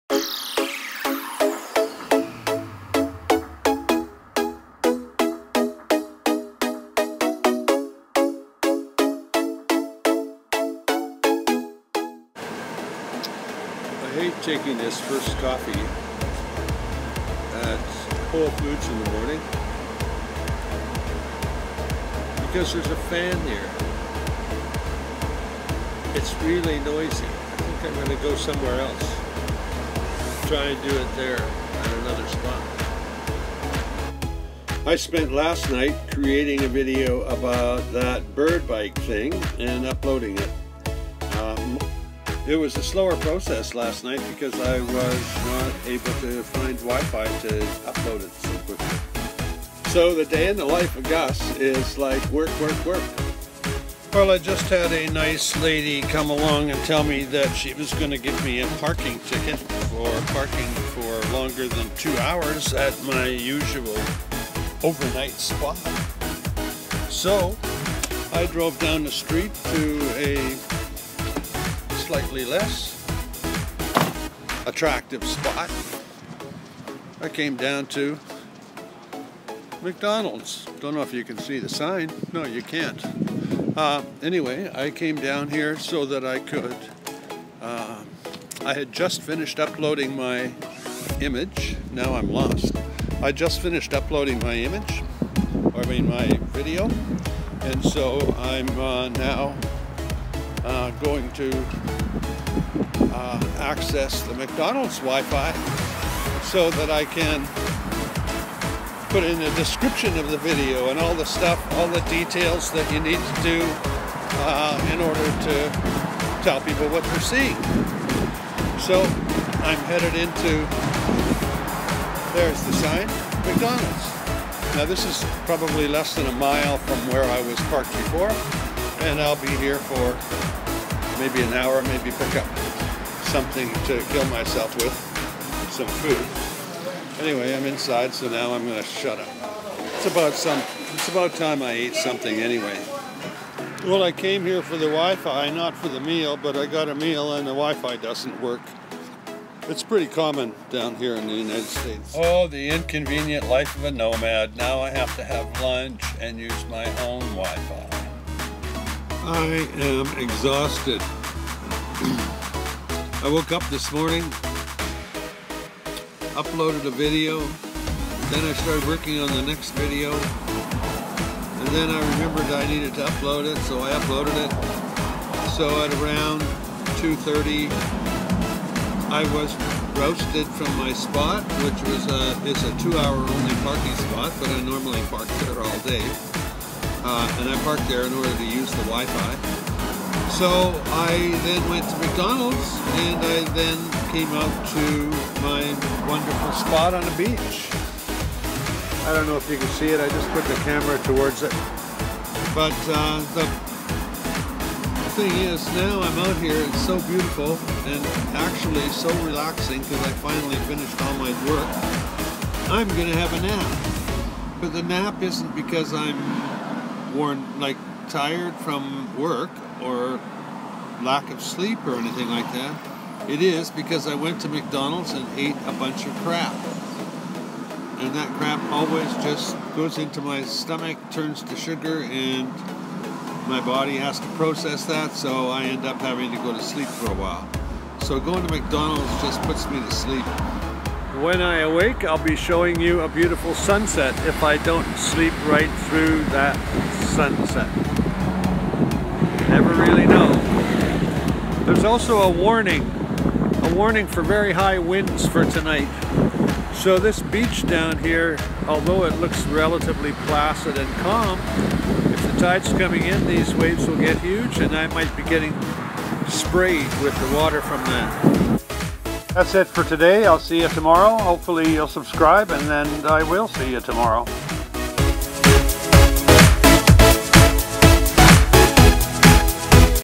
NO parking Ticket - a day of moving around
I avoid a parking ticket and spend the day in paradise

Other LINKS